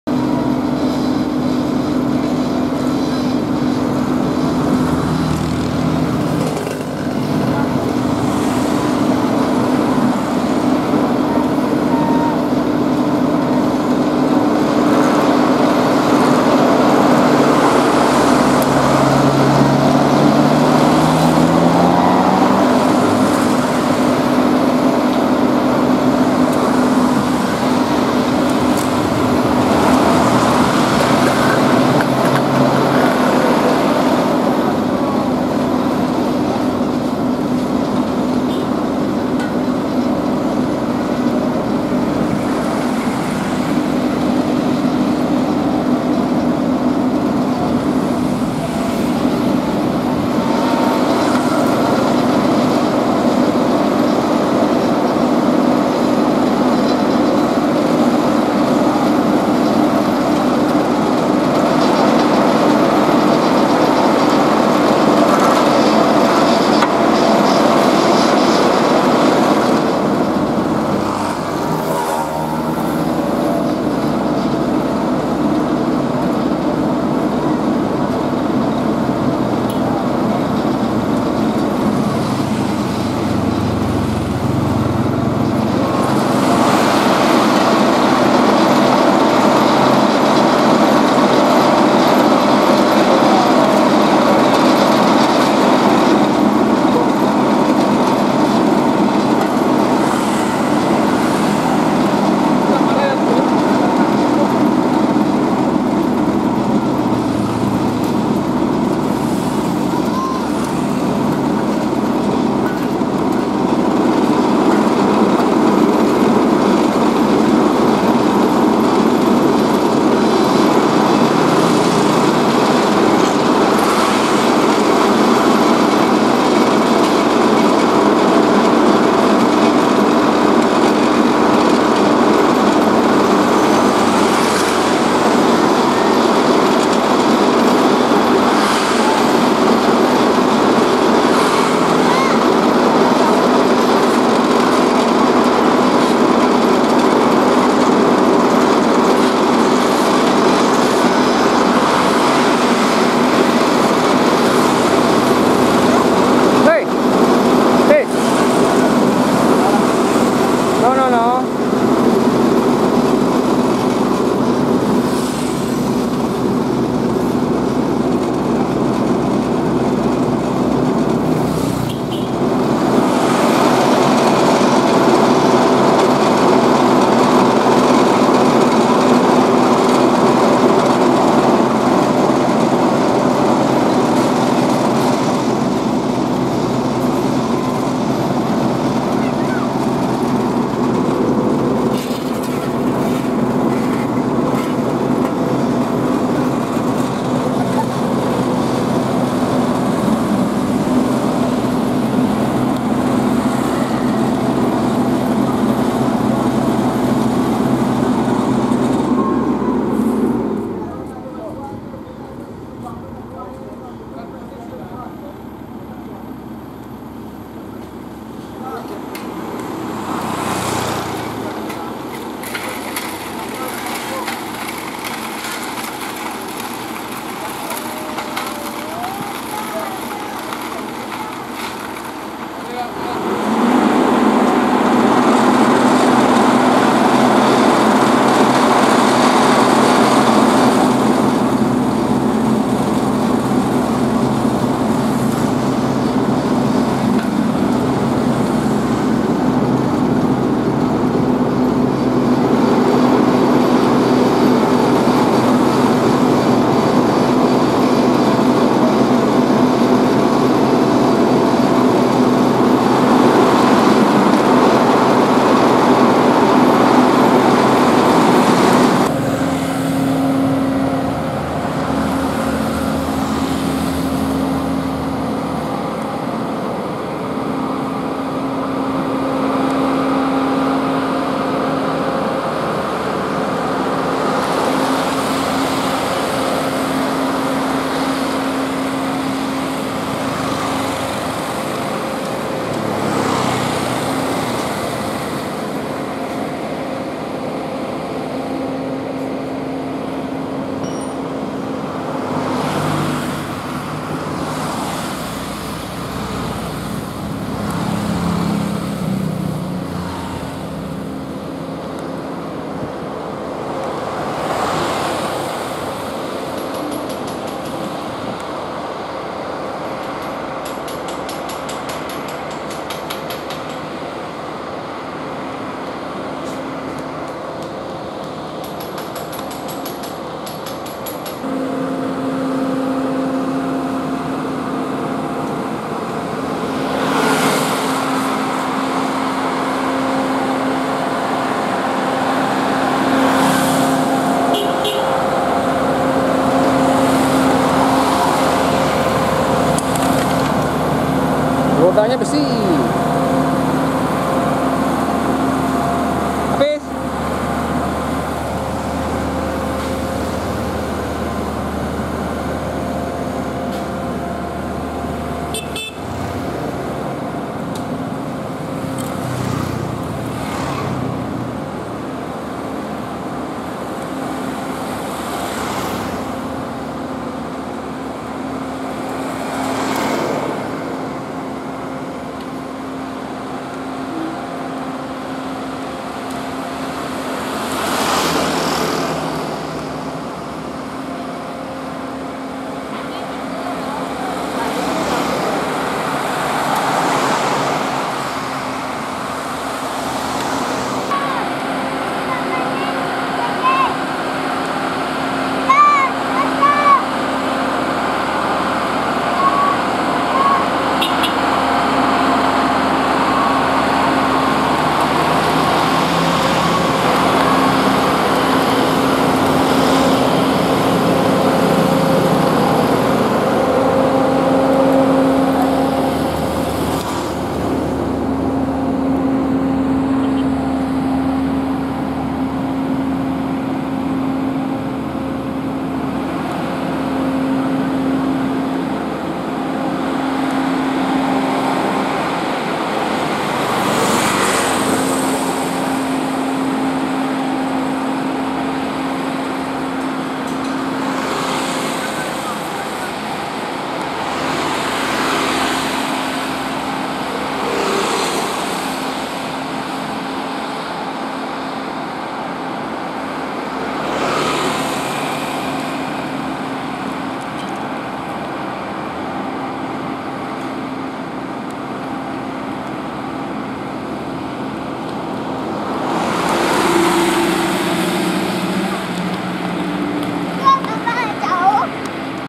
Pengaspalan Jalan Veteran Bukittinggi Menggunakan Dump Truck, Asphalt Finisher, Tandem Roller
Pengaspalan Jalan Veteran, Simpang Jirek - Simpang Tembok,
Kec. Mandiangin Koto Selayan, Kota Bukittinggi, Sumatera Barat, pada Sabtu (27/7/2024).

Pengerjaan dilakukan oleh manusia dibantu Dump Truck, Air Compressor, Asphalt Srayers, Asphalt Finisher, Tandem Roller, dan Pneumatic Tyred Roller.